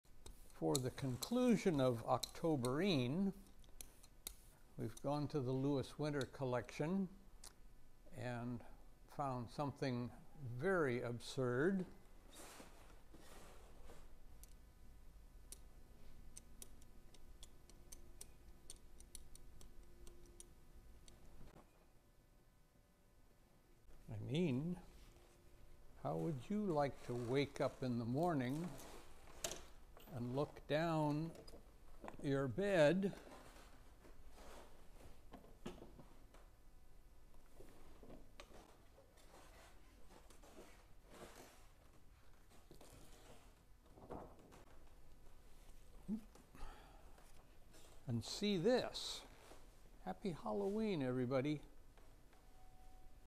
Printing the most absurd cuts from the Lewis Winter Collection for Halloween 🎃 🦶
Happy Halloween! In honor of today, Howard printed two of the most absurd and strangest cuts that we have in our print shop exhibit. These 125-130 year old electrotypes (copies of woodcuts) are from the Lewis Winter Collection.

We are not sure why Lewis Winter designed these but Howard thought to print them facing away from each other.